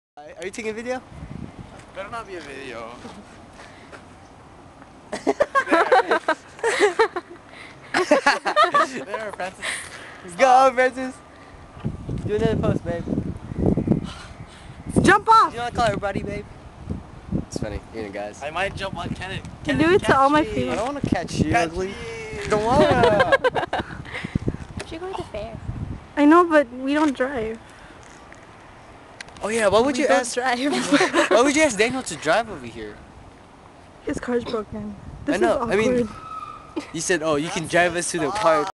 J.Lee On A Swing
jeffrey on a swing, hence the title of this video. haha. listen carefully. "kenneth catch meeeeee"